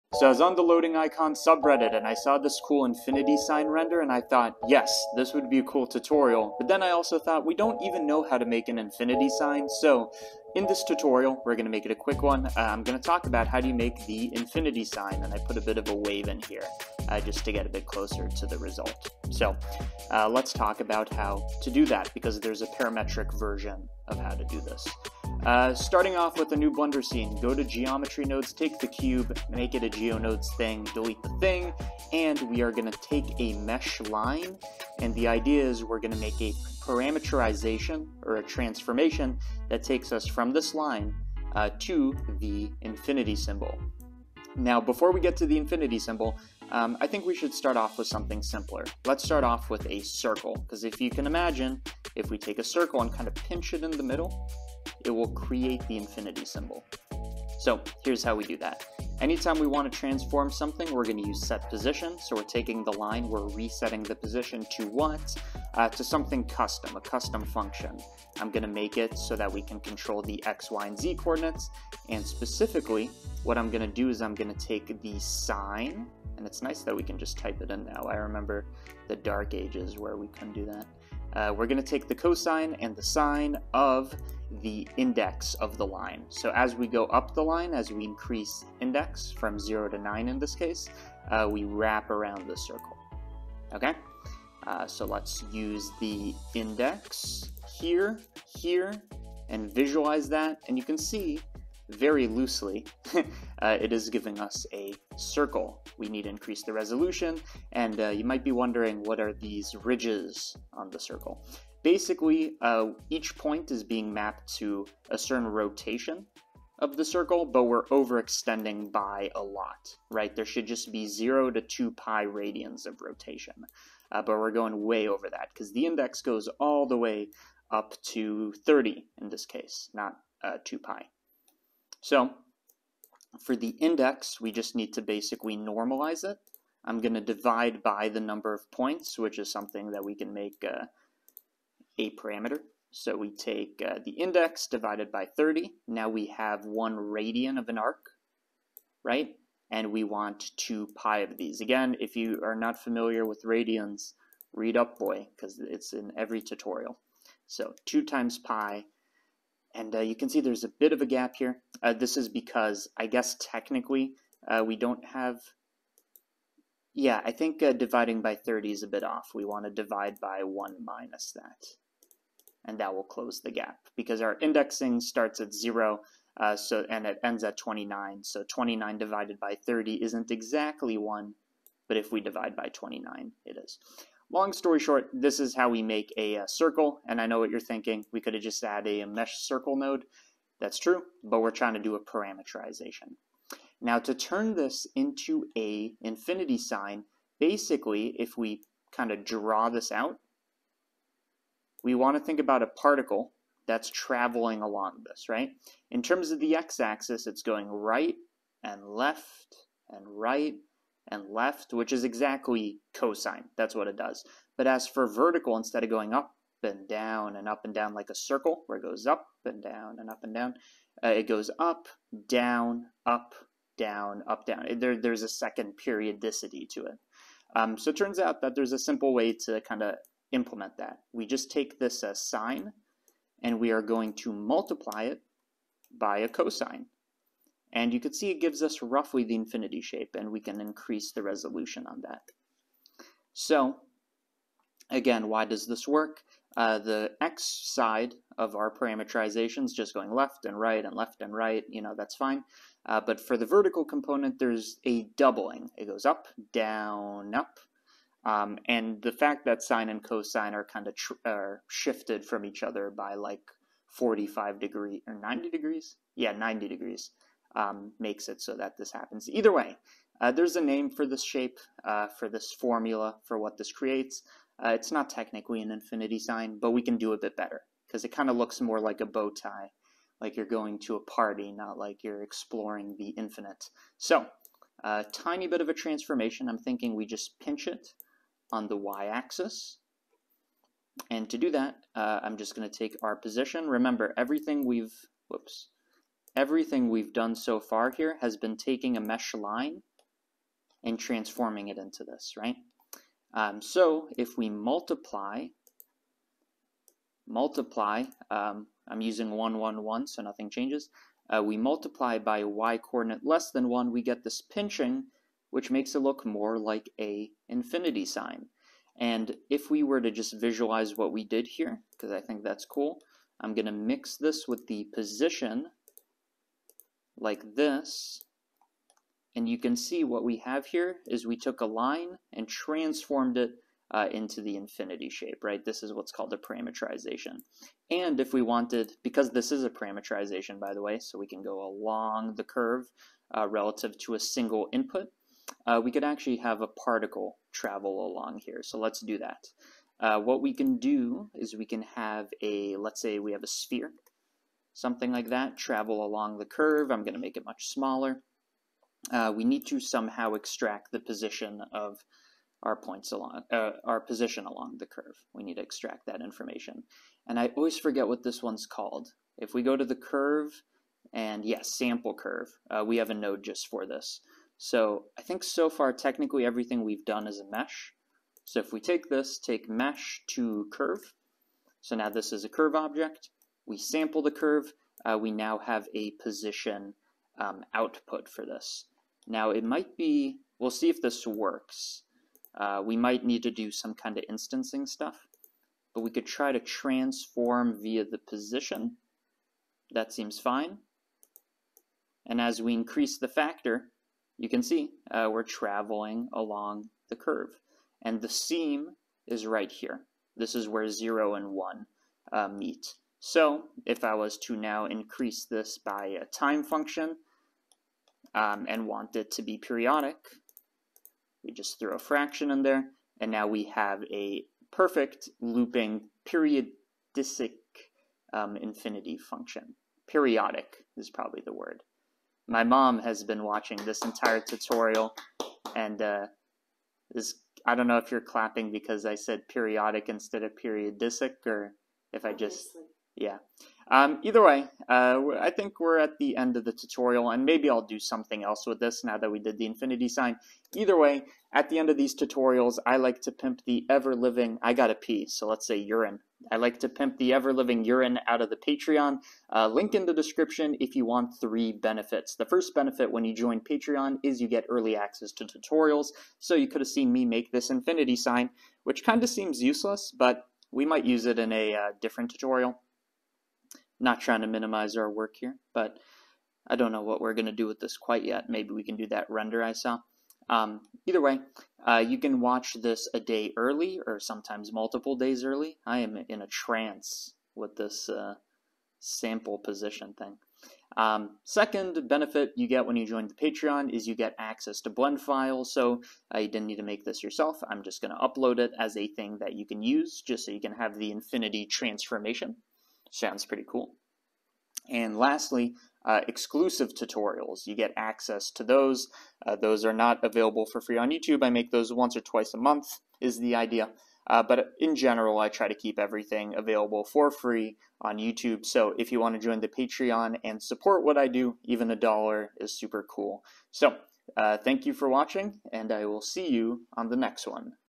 Blender is INFINITE!
We make an infinity sign with blender geometry nodes.